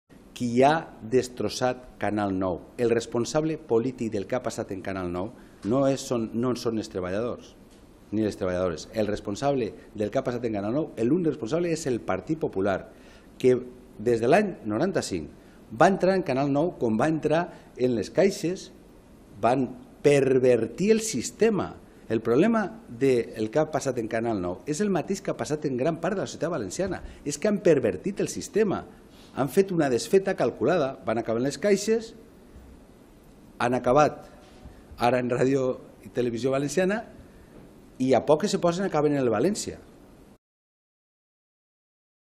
Ximo Puig: "El responsable del que ha passat en Canal 9 és el PP que ha pervertit el sistema"
El secretari general del PSPV-PSOE, Ximo Puig, ha presentat el 'Consell per un nou Canal 9' després de participar en la seua primera reunió de treball. Així, ha explicat que aquest consell està format per persones de diferents àmbits, és "un consell transversal i obert" amb l'objectiu de configurar "una posició compartida" amb la majoria de la societat i també amb els partits polítics, que han presentat una proposta de llei en Les Corts. Sobre la viabilitat econòmica, ha reiterat que la Comunitat necessita una radiotelevisió pública viable i ha acusat al PP de ser el responsable del que ha passat en Canal 9. "El PP ha pervertit el sistema", ha indicat.